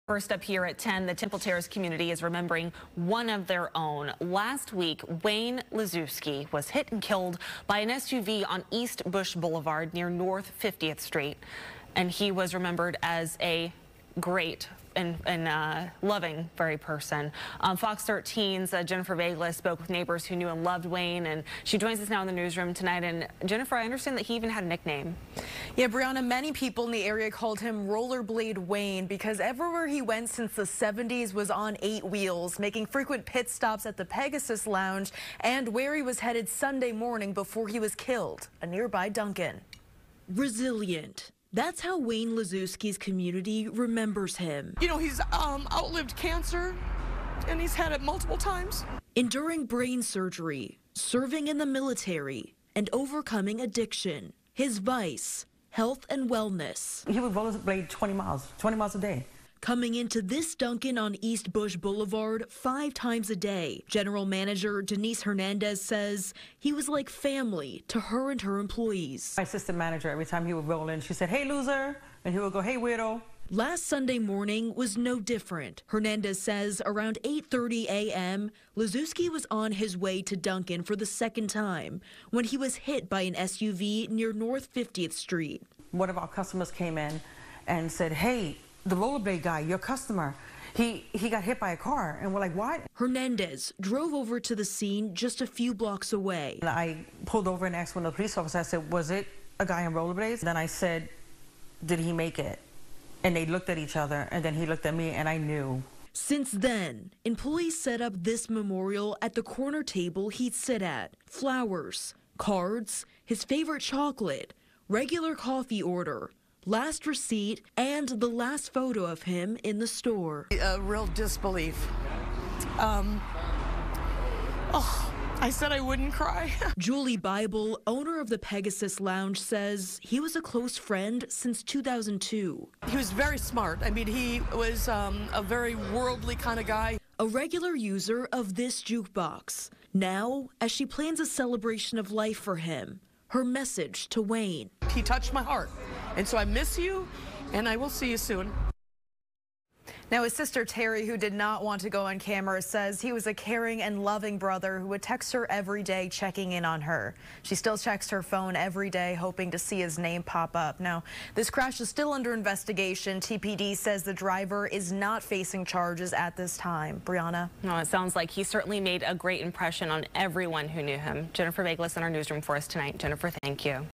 Florida man who beat cancer, addiction killed while rollerblading
A Florida community is grieving the loss of a beloved rollerblader who was hit and killed by an SUB after beating cancer and addiction.